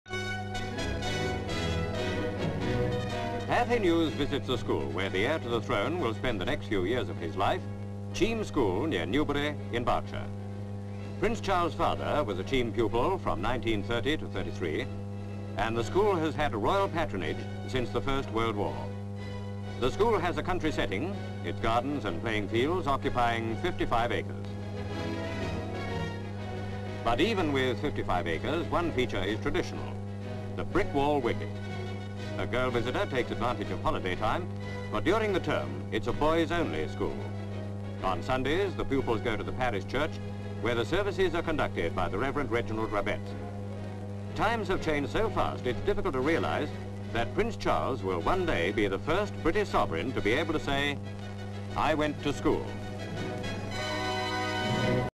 Prince Charles' New School (1957)
This is a duplicate copy of item 1499.12 - check for best quality.

Headley, Berkshire.

GV. Big, of the Prince Charles' new school - Cheam School, Headley. GV. School with tennis court in foreground. SV. Showing front door of the school. GV. Buildings showing dormitories and classrooms. SV. Interior of room showing three made up beds without any linen on them. SV. Showing three more beds with clothes on them. GV. Showing school and house - the gardener mowing in the foreground. Pan with gardener to show large playing fields. GV. Classrooms and two children playing cricket, stumps painted on wall. CU. Little girl bowls the ball. Young man hits the ball. Showing painted cricket stumps on wall. GV. Showing swimming pool in foreground and children playing on the spring board. GV. Old church. SV. Towards and pan, the Reverend Rabbetts goes towards church doorway. GV. Top, sunken garden with beautiful lawns flanking it. GV. On the ground, of the sunken garden. GV. The school.

(Neg.) (Title Scene E)

Note: this is the last item on tape PM3456 - tape ends at 01:42:49.
 FILM ID:3456.22